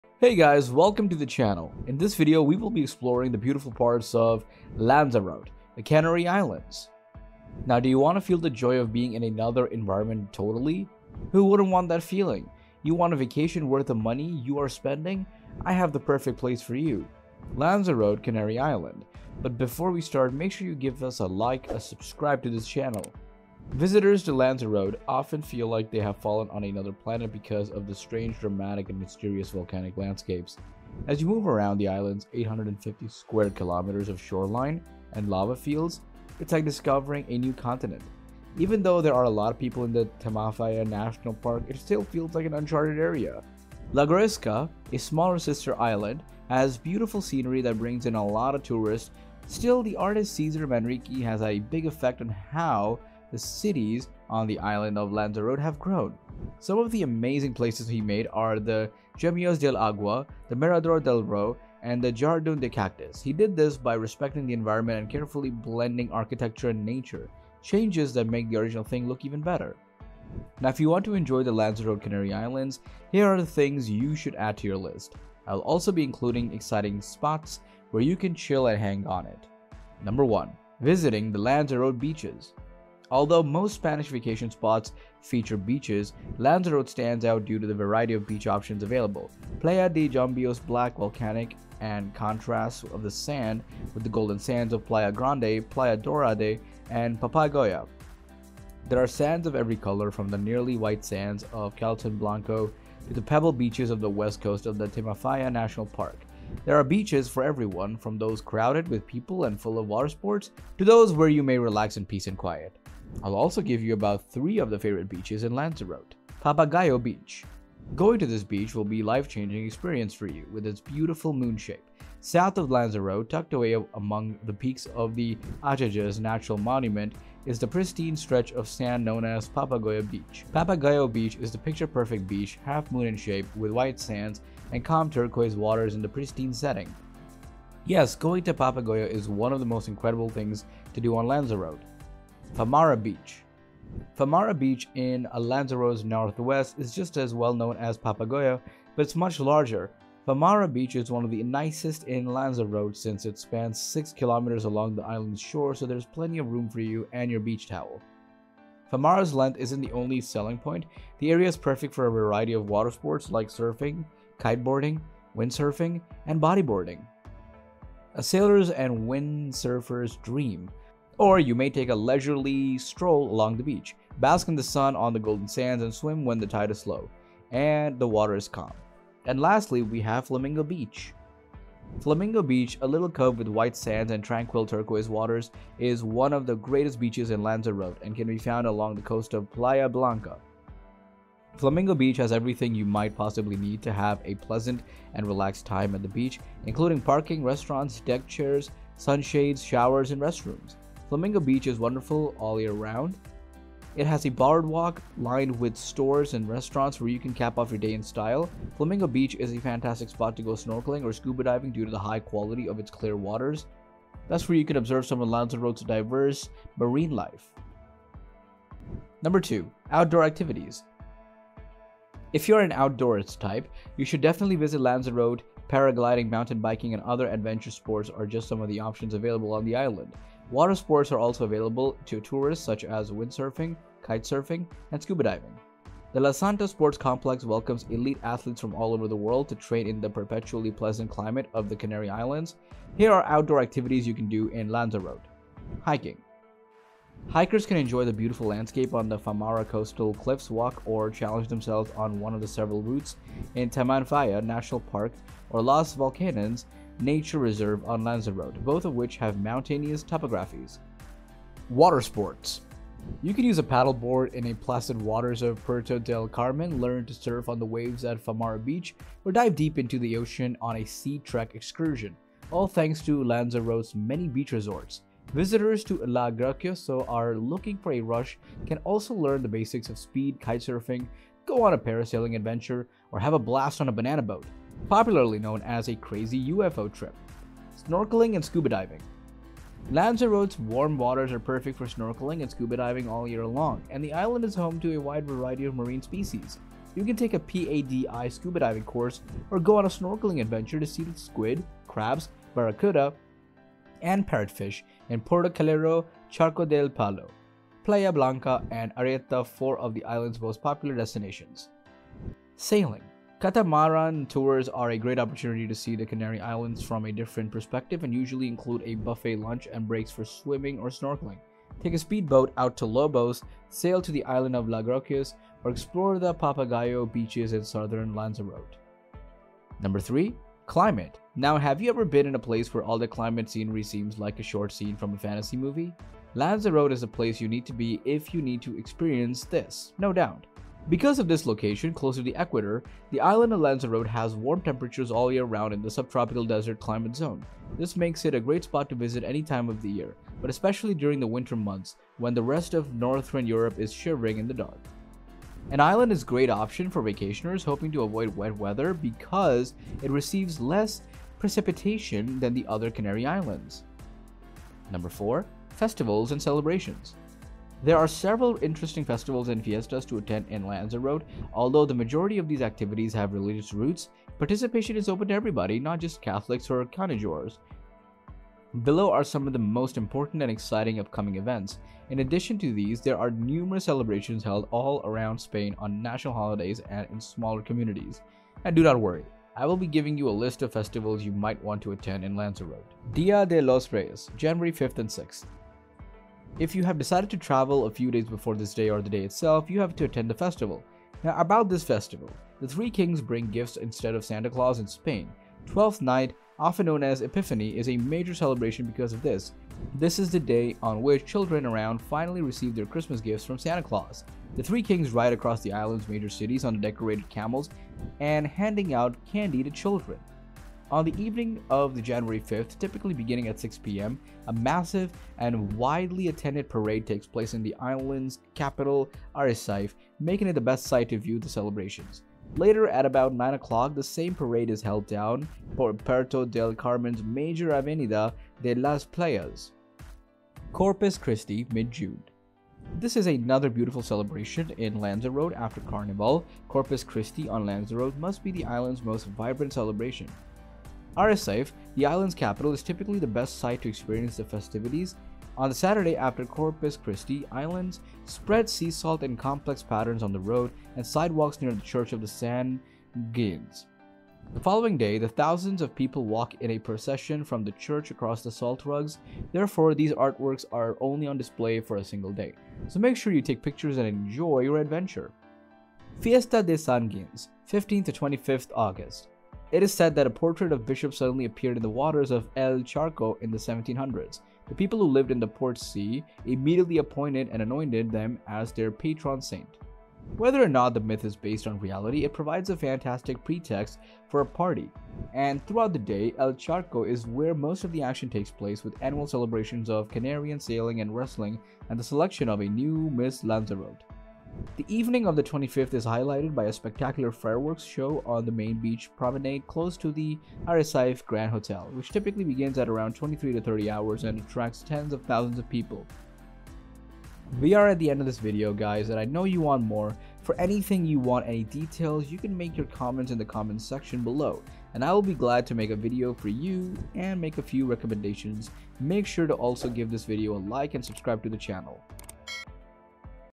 Explore Lanzarote: A Mysterious Canary Island Awaits!
Hey, guys, welcome to this channel. In this video, we will be exploring the beautiful parts of Lanzarote, the Canary Islands.

Now do you want to feel the joy of being in another environment totally? Who wouldn't want that feeling? You want a vacation worth the money you are spending. I have the perfect place for you. Lanzarote, Canary Island.

Visitors to Lanzarote often feel like they have fallen on another planet because of the strange, dramatic, and mysterious volcanic landscapes. As you move around the island's 850 square kilometers of shoreline and lava fields, it's like discovering a new continent. Even though there are a lot of people in the Timanfaya National Park, it still feels like an uncharted area.

La Graciosa, a smaller sister island, has beautiful scenery that brings in a lot of tourists. Still, the artist César Manrique has had a big effect on how the cities on the island of Lanzarote have grown. Some of the amazing places he made are the Jameos del Agua, the Mirador del Ro, and the Jardn de Cactus. He did this by respecting the environment and carefully blending architecture and nature. Changes that make the original thing look even better.

Now if you want to enjoy Lanzarote, Canary Islands, here are the things you should add to your list. I will also be including exciting spots where you can chill and hang out in.

00:00 Introduction
01:20 Visiting the Lanzarote Beaches
01:56 Papagayo Beach
02:22 Famara Beach
03:00 Flamingo Beach
03:39 Outdoor Activities
04:05 Hiking
04:21 Water Sports
04:47 Snorkeling and Scuba Diving
05:12 Sailing
05:30 Climate
06:05 Festivals and Celebrations
06:33 Dia De Los Reyes
07:21 Corpus Christi
07:56 Fiesta de San Gines